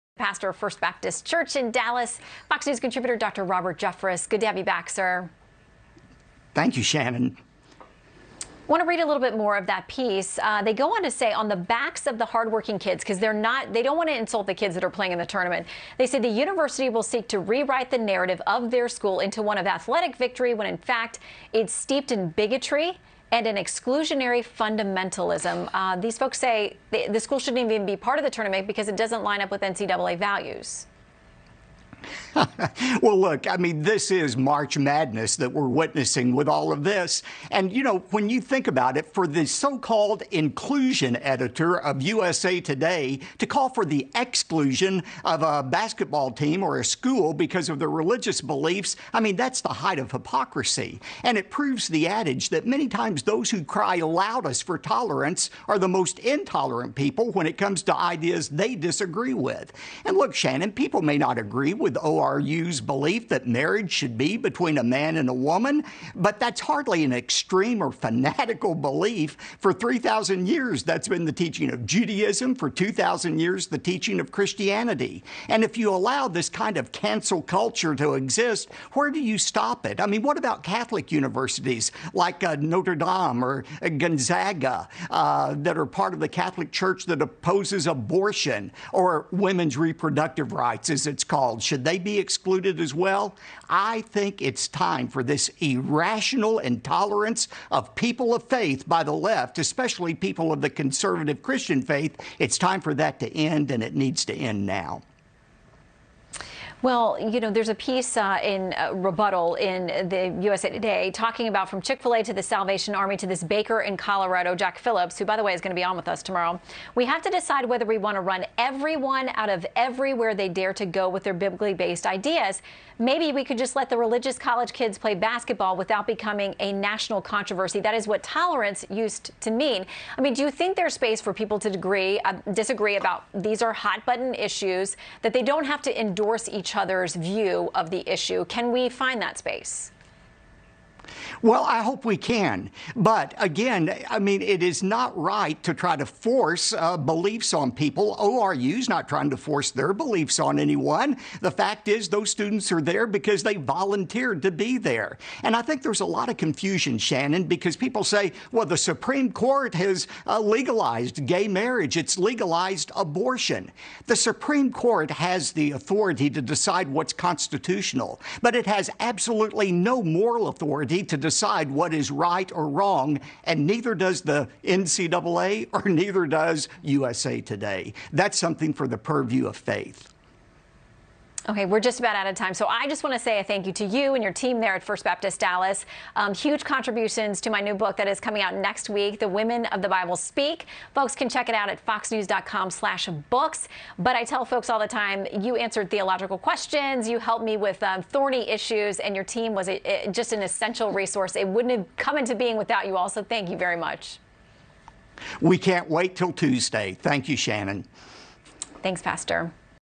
USA Today cere excluderea unei echipei de baschet din cauza convingerilor lor religioase
USA Today cere excluderea unei echipe de baschet sau a unei școli din cauza convingerilor lor religioase, este apogeul ipocriziei. Și dovedește zicala că de multe ori cei care strigă cel mai tare pentru toleranță sunt cei mai intoleranți, când vine vorba de idei cu care nu sunt de acord.

Oamenii pot să nu fie de acord cu convingerile Universității Oral Roberts despre căsătoria dintre bărbat și o femeie, dar asta nu este o credință extremă sau fanatică. De 3000 ani aceasta este învățătura iudaismului și de 2000 ani a creștinismului. Și dacă lăsăm cancel culture să existe, mă întreb unde o putem opri? Ce spui de universitățile catolice precum Notre Dame sau Gonzaga, care fac parte din Biserica Catolică, care se opune avortului, drepturile de reproducere ale femeilor, așa cum sunt numite?

Ar trebui excluse și ele? Cred că este timpul ca această intoleranță irațională a celor de stânga față de credincioși, în special față de creștinii conservatori, să se termine și trebuie să se termine acum.